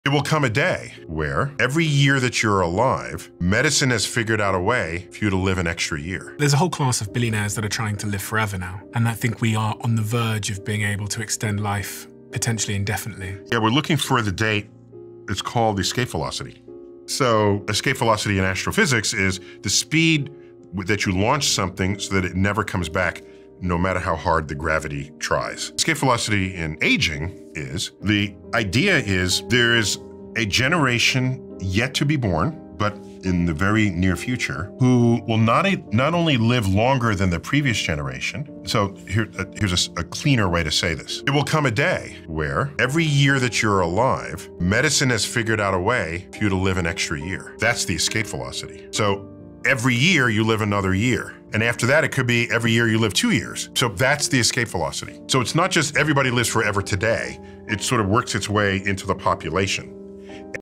Living Forever with Medical Advancements
Imagine a future where medicine adds a year to your life for every year you live. Billionaires are chasing immortality. Will we achieve 'escape velocity' in aging?